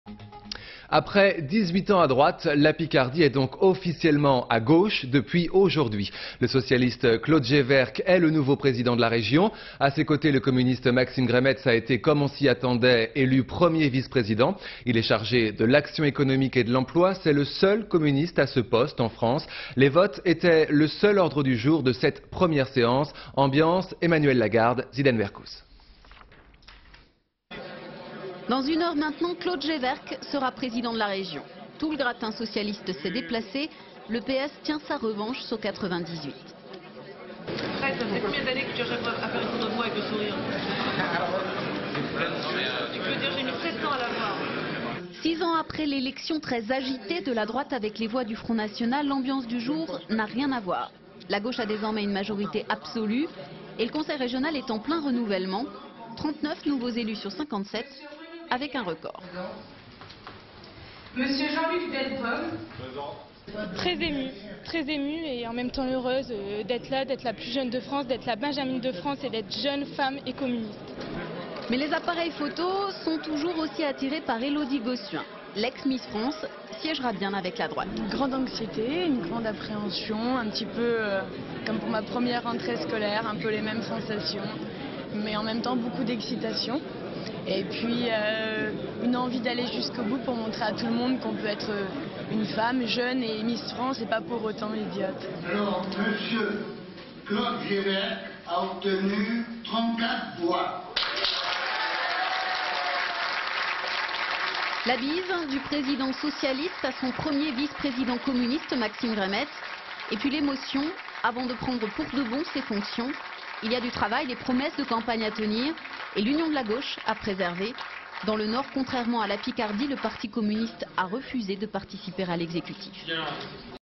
Election de Claude GEWERC au conseil régional
2 avril 2004
 Après 18 ans à droite, la Picardie est officiellement à gauche. Le socialiste Claude GEWERC est le nouveau président de la région. A ses côtés, le communiste Maxime GREMETZ a été, comme on s'y attendait, élu premier vice président. Il est chargé de l'action économique et de l'emploi. C'est le seul communiste à ce poste en France. Les votes étaient le seul ordre du jour de cette première séance. politique; archive television; archive tv; ina; inna; Institut National de l'Audiovisuel; french tv Images d'archive INA